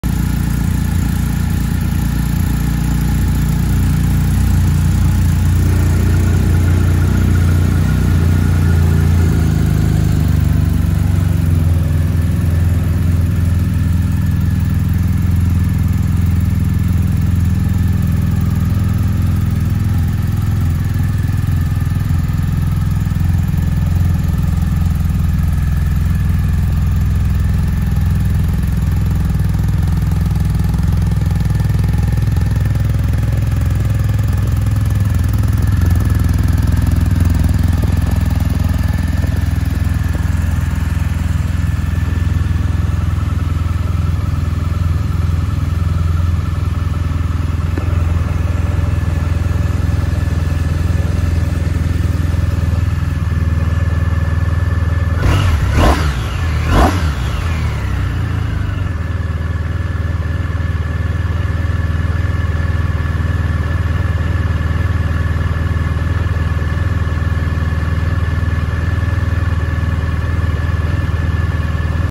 2020 BMW R1250RS Walk Around Video!
Take a moment to do a 360 walk around on our freshly received 2020 BMW R1250RS in Black Storm Metallic!